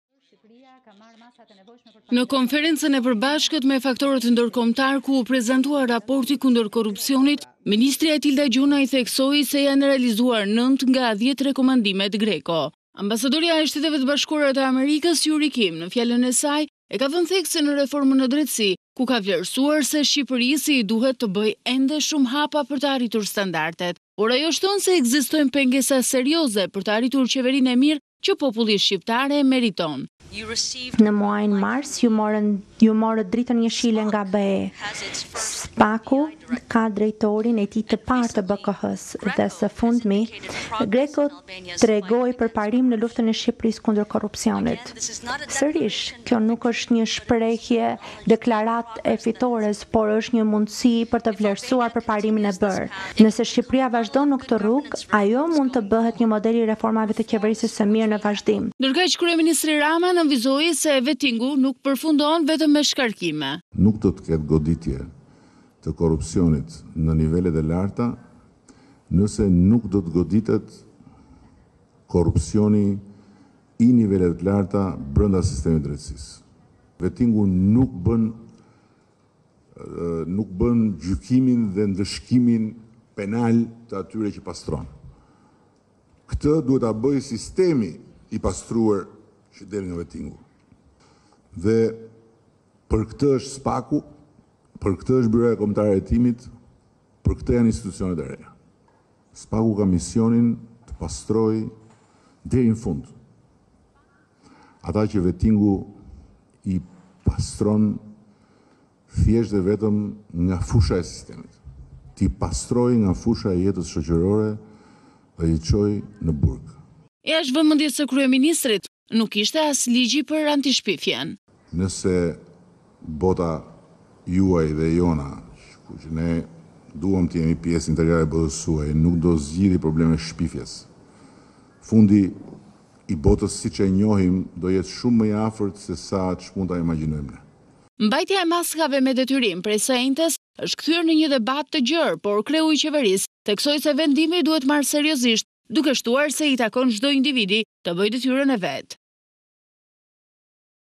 Ora News- Yuri Kim: Ka pengesa serioze për qeverinë e mirë, zyrtarë të korruptuar, drejtësi të kapur
Në konferencë e nivelit te larte antikorrupsion ambasadorja e SHBA-së Yuri Kim nënvizoi se ekzistojnë pengesa serioze për të arritur qeverinë e mirë që populli shqiptar e meriton

Yuri Kim: Qytetarët e Shqipërisë si të gjithë qytetarët e botës meritojnë të kenë besim se paratë që vijnë nga djersa e punës së tyre po përdoren për të përmirësuar jetën e tyre të përditshme.

Kur shikoj reformën në drejtësi në  Shqipëri është një çështje që ka të bëjë  me të vendosurit një këmbë pas tjetrës. Në një kohë historike, në një mal që është po aq i thepisur sa mund të jetë. Sikurse Luigji dhe të tjerë e kanë thënë që korupsioni është një problem i madh është një mal problemesh në Shqipëri, por hap pas hapi ne jemi duke bërë përparim.

Ekzistojnë pengesa serioze për të arritur qeverinë e mirë që populli shqiptar e meriton. Këto pengesa përfshijnë zyrtarë të korruptuar, një sektor drejtësie të kapur nga interesa politikë, gazetarë të bërë palë ose janë këcënuar për raportimet që bëjnë, ndërmarrje shtetërore që i japin përparësi klientelizmit ose miqësive mbi operacionet e efektshme dhe zyrtarë politik të interesuar në nxitje të interesit vetijak në vend që të mund të bëjnë diçka të mirë për vendin.